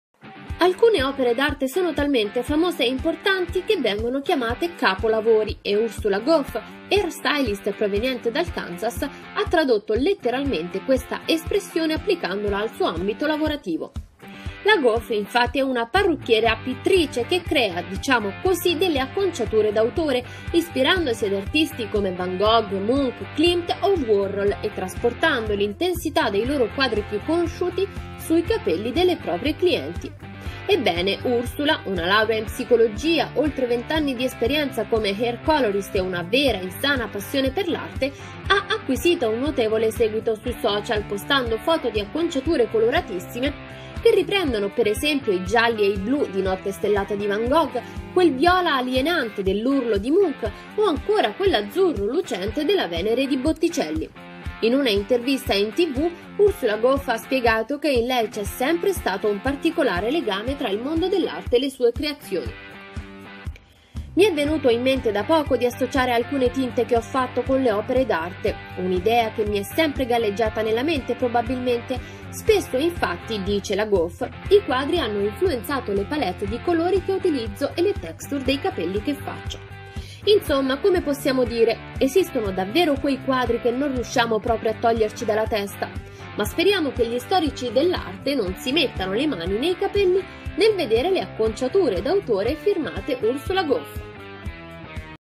Ursula Goff, la parrucchiera-pittrice che crea capelli d’autore
Dal Kansas Ursula Goff è diventata famosa per ricreare acconciature coloratissime ispirandosi a capolavori dell’arte come ‘Notte stellata’ di Van Gogh